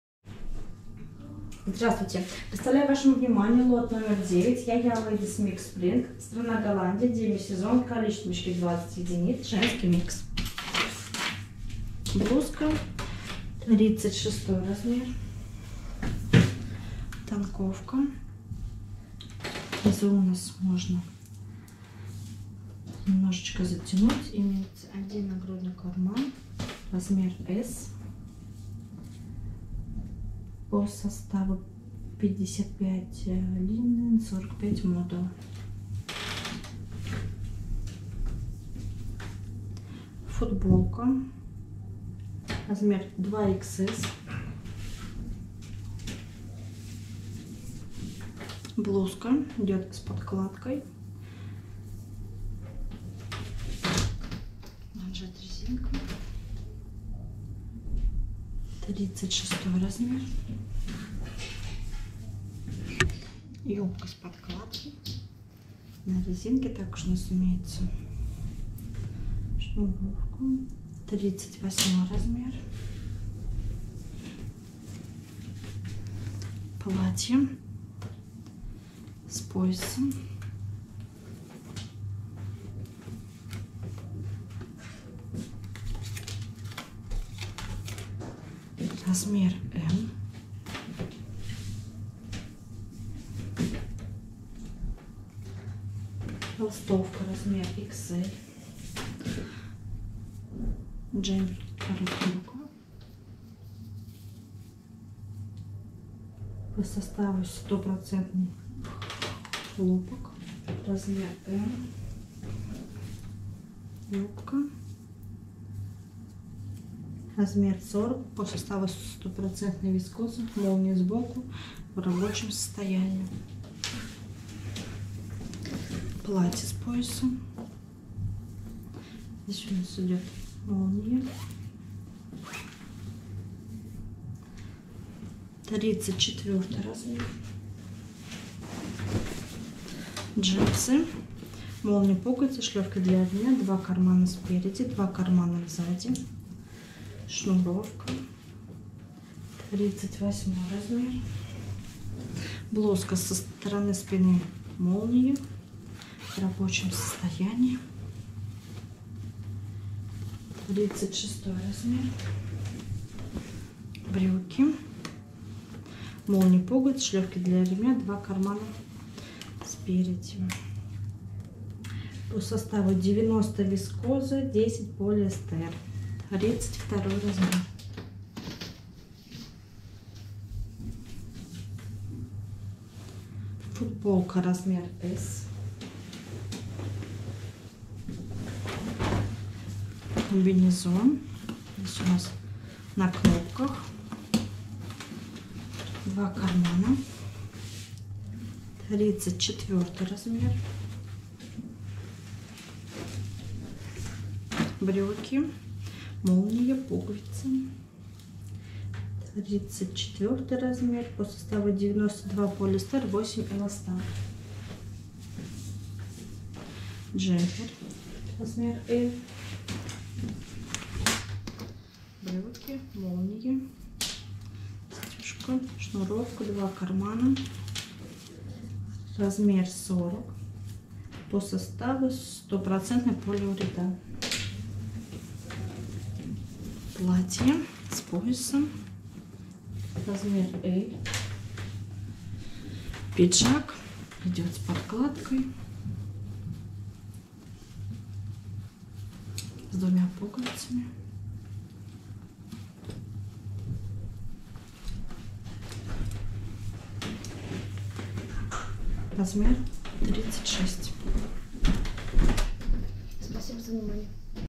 YAYA LADIES MIX SPRING 9 ,Цена за ед : 9.39 евро, сток одежда оптом
YAYA LADIES MIX SPRING (лот № 9)
Женский микс (сток)
Страна: Голландия
Сезон: Весна
Цена за мешок: 187.80 евро
Кол-во в мешке ,шт.: 20
Цена за ед.: 9.39  евро